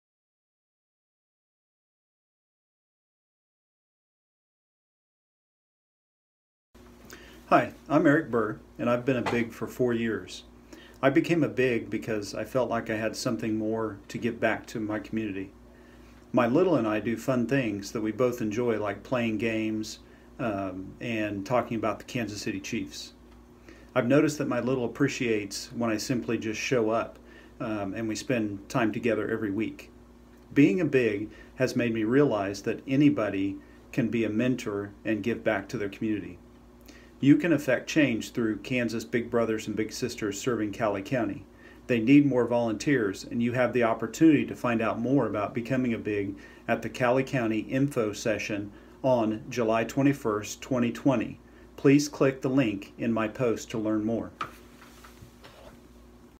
Big Brother Captain Eric Burr
Captain Eric Burr has been matched with his Little Brother for 4 years! He became a Big to give back even more to his community.

You too can affect change in Cowley County.  Join us on Tuesday, July 21st @ 12pm for our virtual Info Session to learn more!